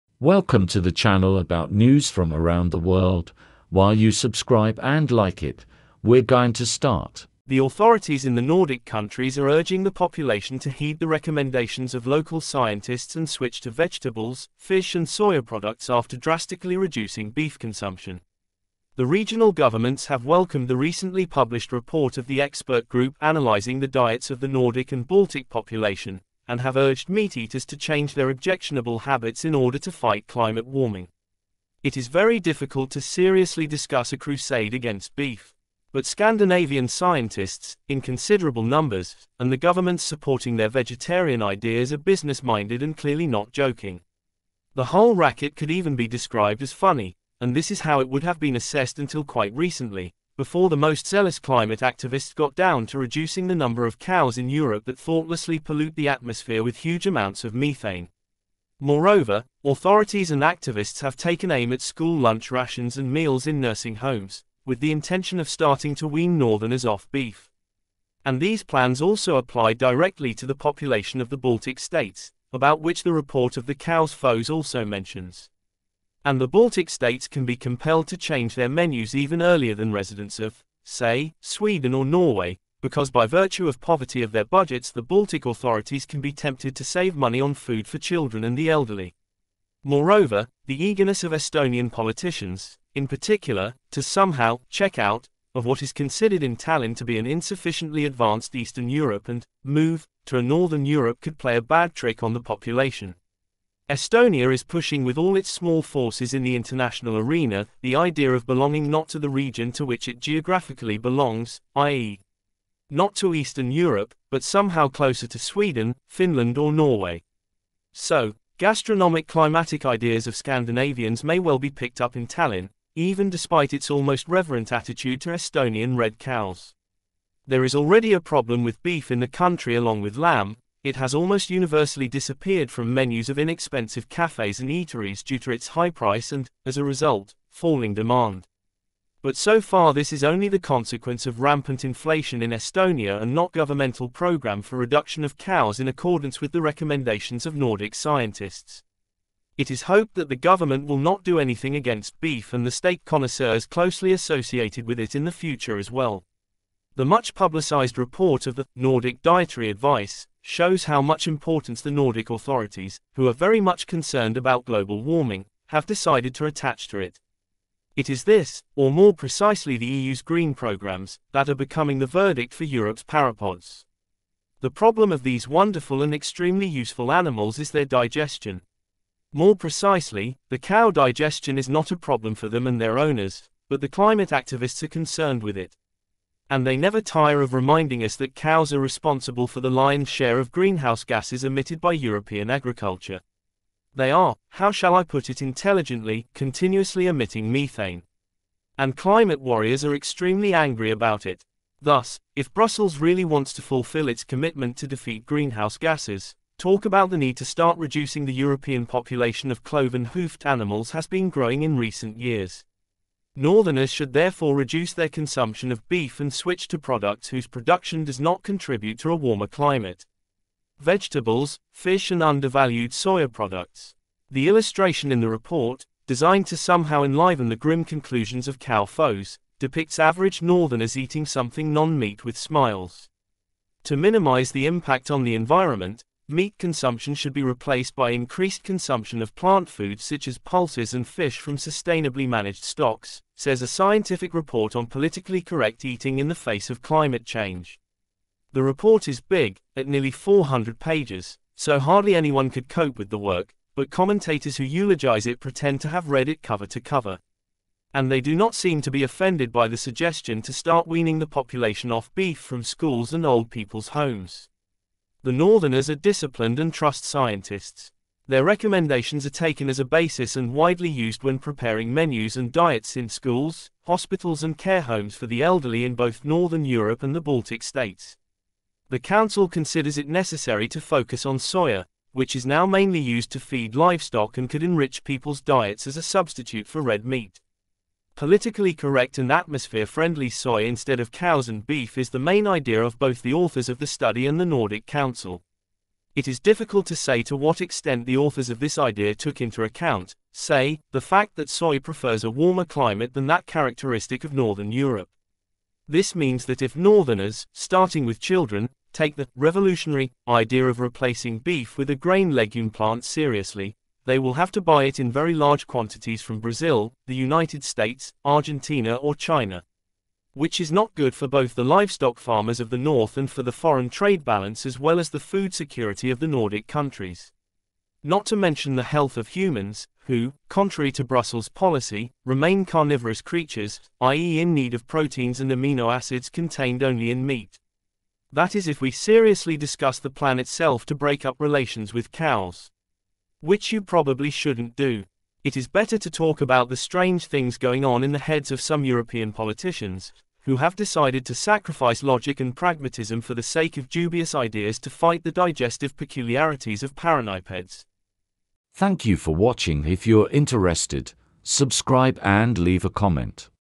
What to show? European cows... Don't fit in with the 'green' agenda...
The authorities in the Nordic countries are urging the population to heed the recommendations of local scientists and switch to vegetables, fish and soya products after drastically reducing beef consumption. The regional governments have welcomed the recently published report of the expert group analysing the diets of the Nordic and Baltic population, and have urged meat-eaters to change their objectionable habits in order to fight climate warming.